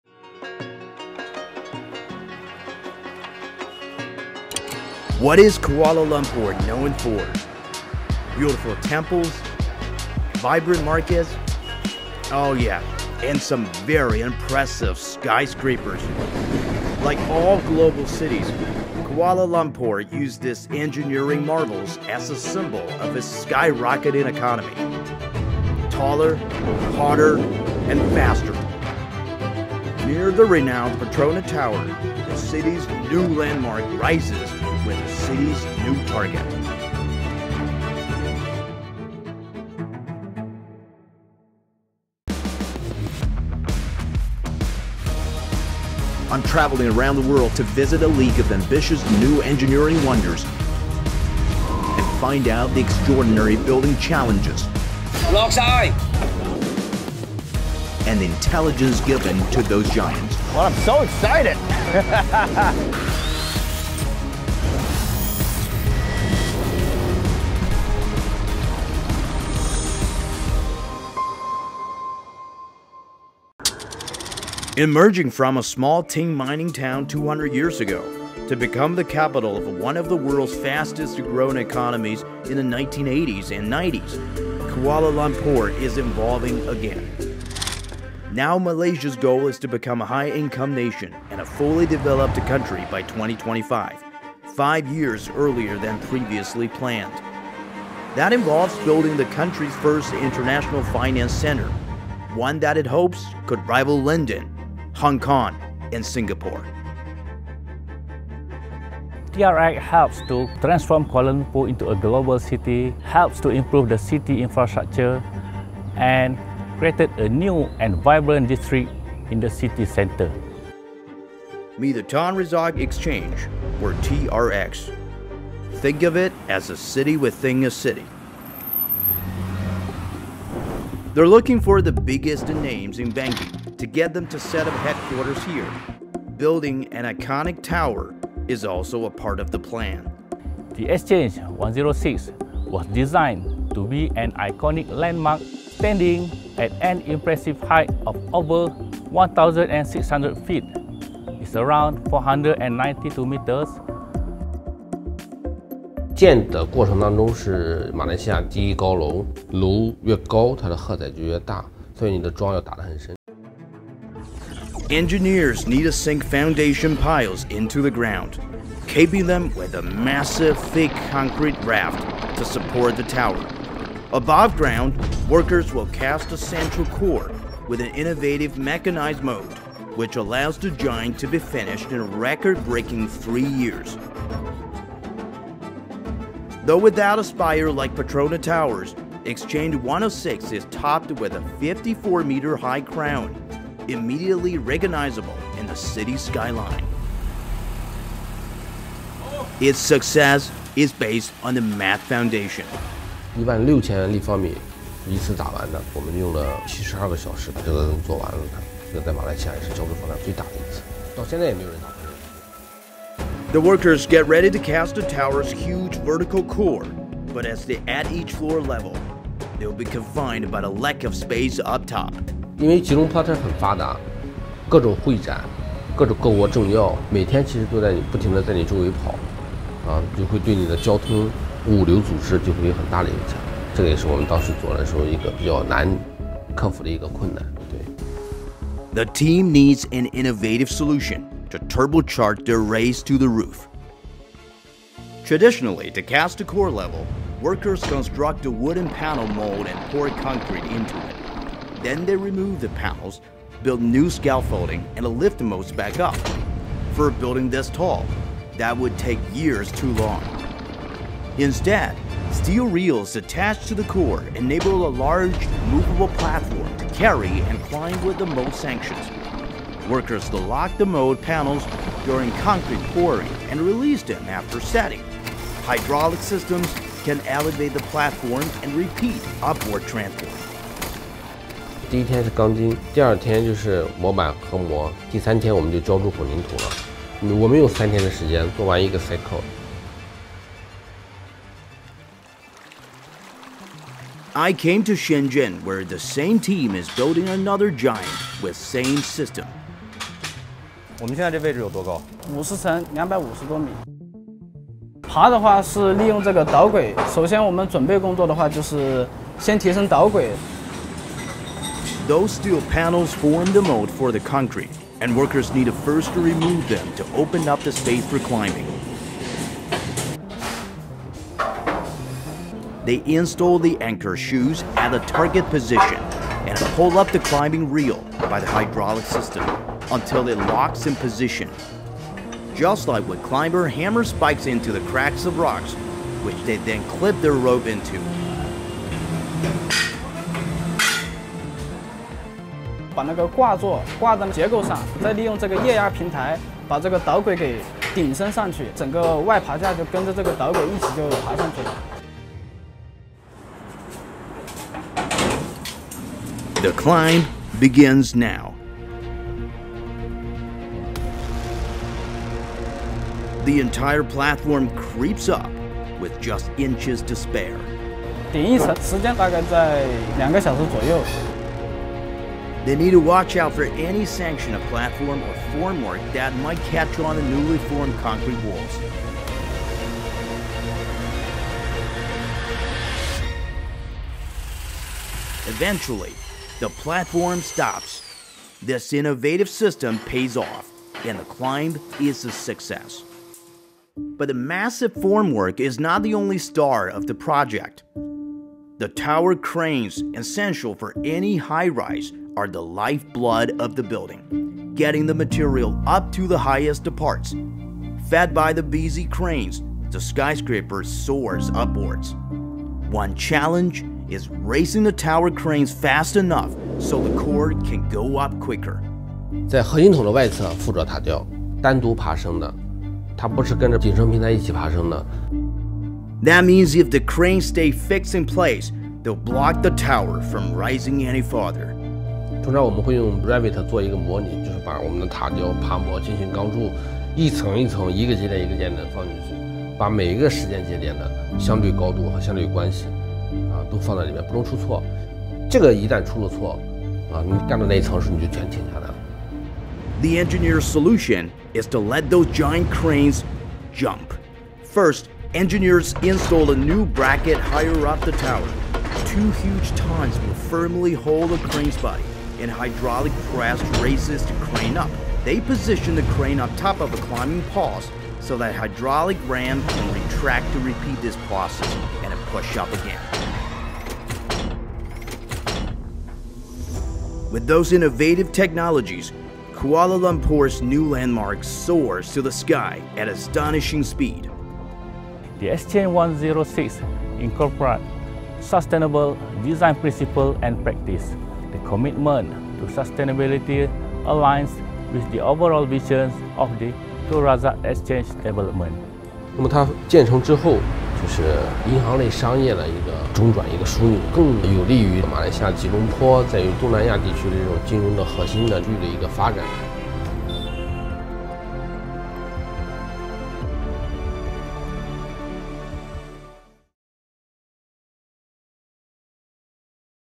Architecture Intelligence: How fast can we build a skyscraper? (Exchange 106, Kuala Lumpur)
What is Kuala Lumpur known for? Beautiful temples, vibrant markets and some very impressive skyscrapers. Located just 2.5 kilometers from the iconic Petronas Towers, the city's new landmark rises alongside its ambitious aspirations. @Tech It Out Studio